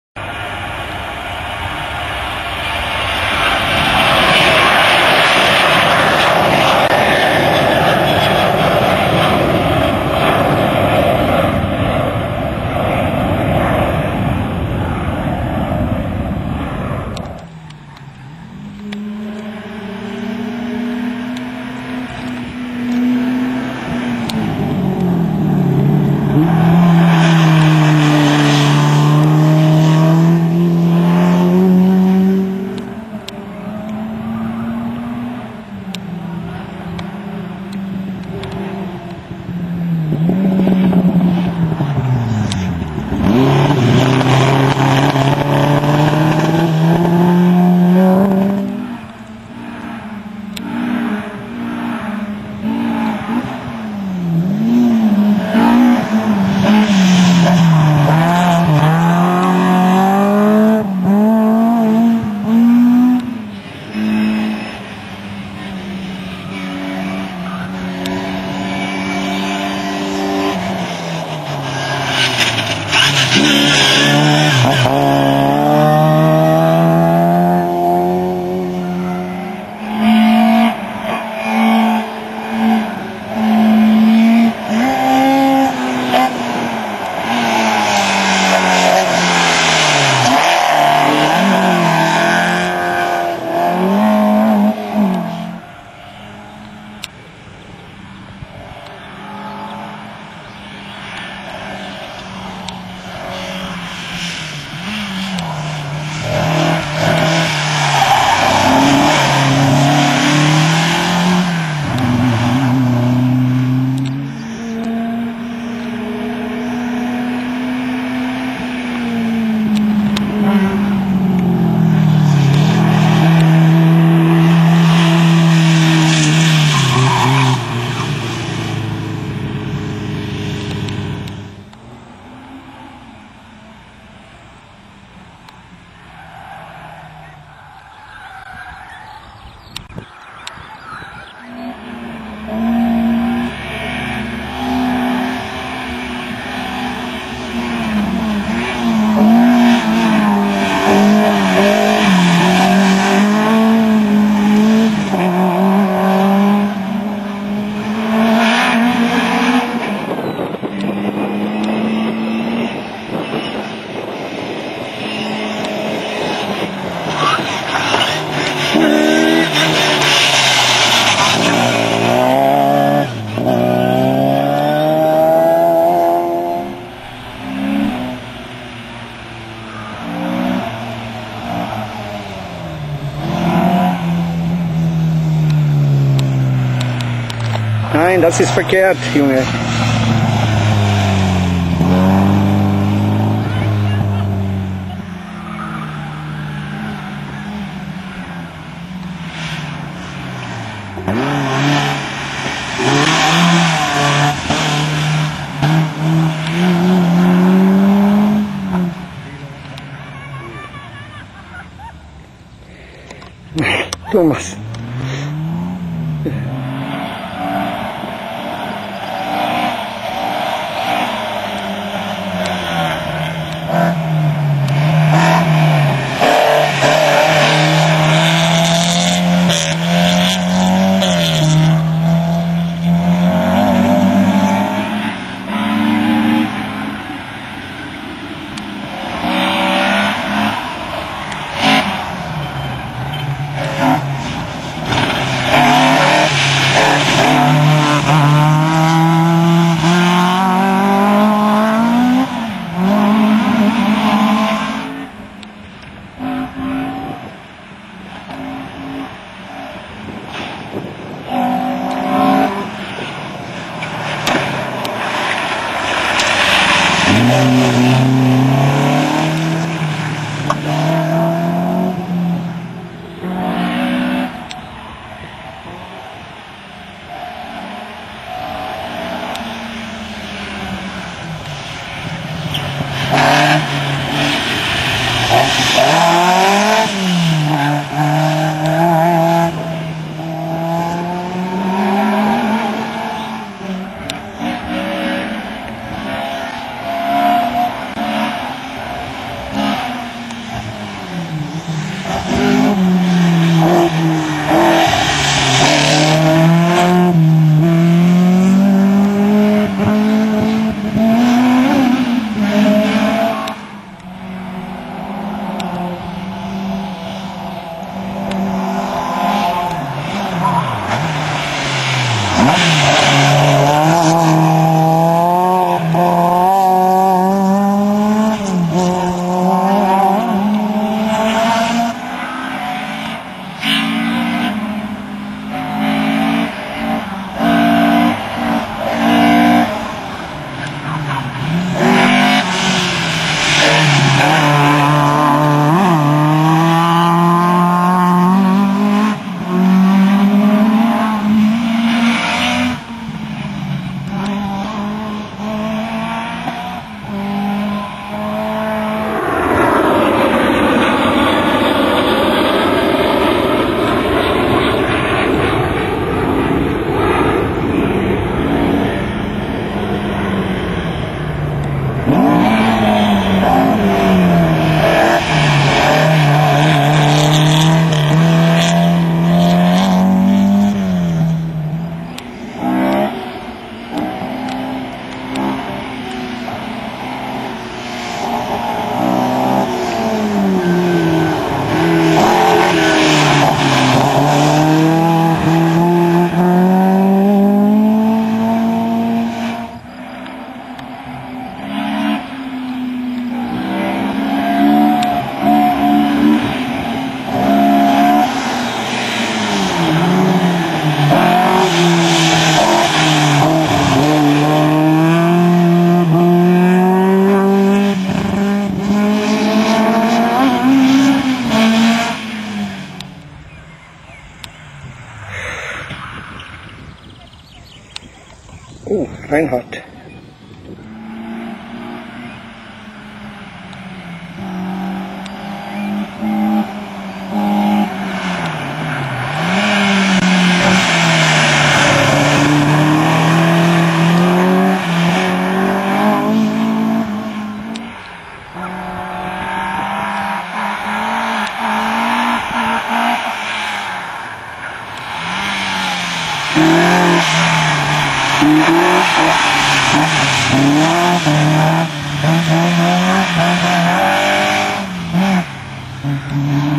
Vorschaumix Weeze, 12.06. 2010 Slalom DM Gr. H und FS - Action
Vorschau aus 2009, diesmal werden Gruppen H und FS gezeigt (wie schon im Mix 1 - das letzte Fahrzeug ...)

12. Juni 2010 - eine tolle Veranstaltung, deutsche Meisterschaft, anschliessend Clubsport, alle Infos : Motorsportfreunde-Solingen.de

kommt einfach vorbei, es lohnt sich !